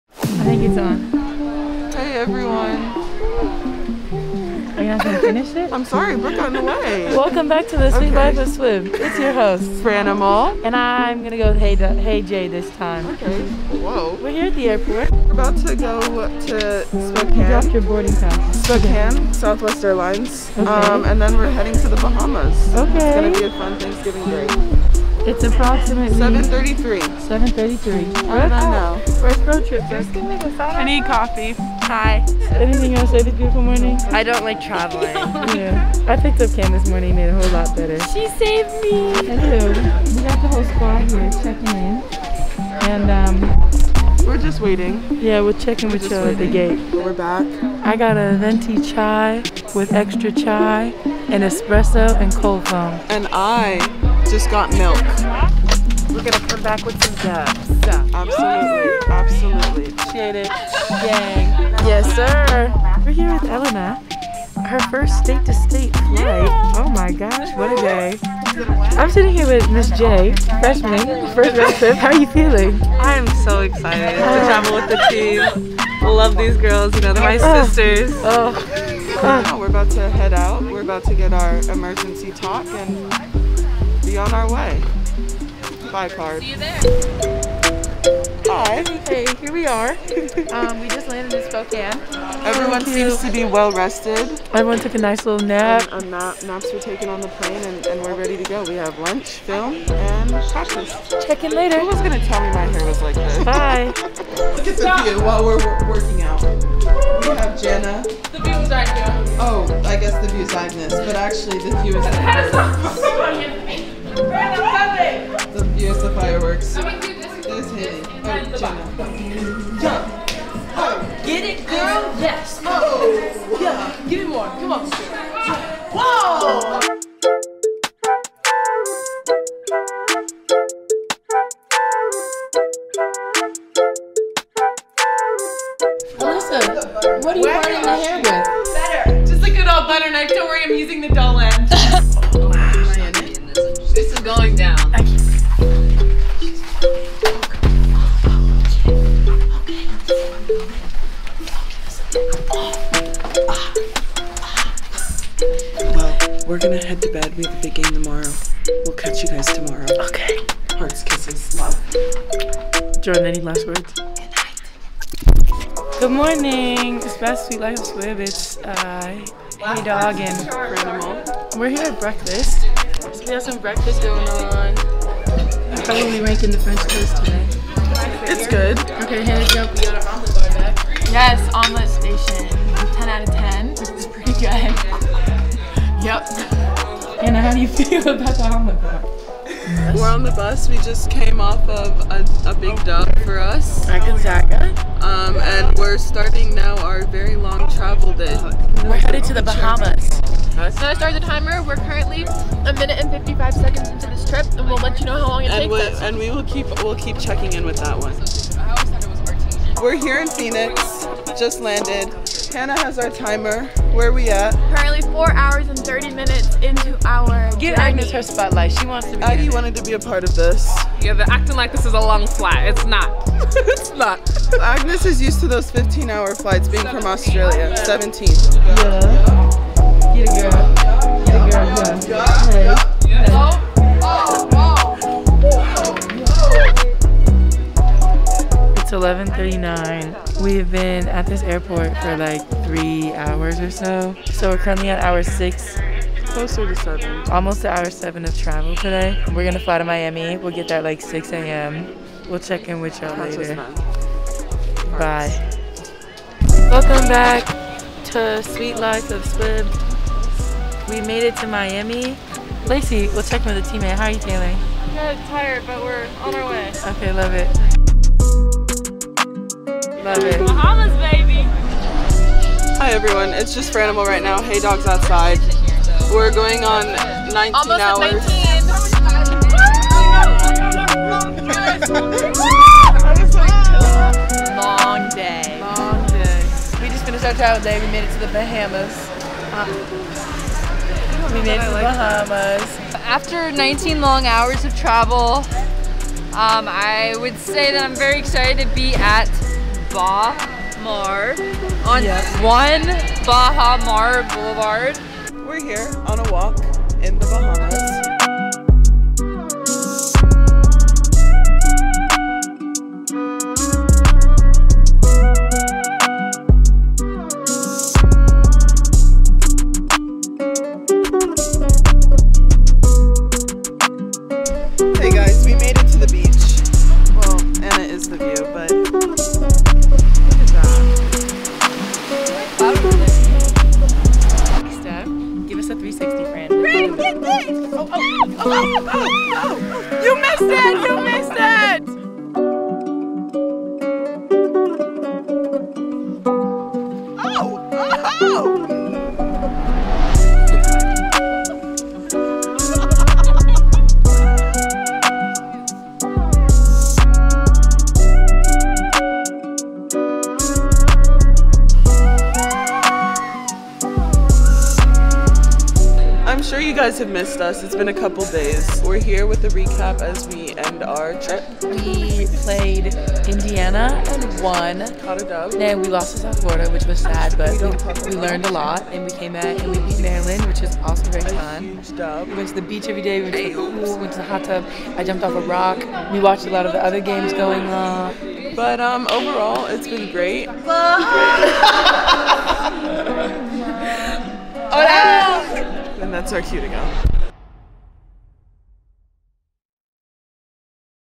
The Sweet Life of Stanford Women's Basketball Vlog: Episode 2
Haley, Fran, and the Card are back for Episode 2! Get an inside look at the team's travels to Spokane, Washington and the Bahamas.

Twitter.com/GoStanford
Instagram.com/GoStanford
Facebook.com/StanfordAthletics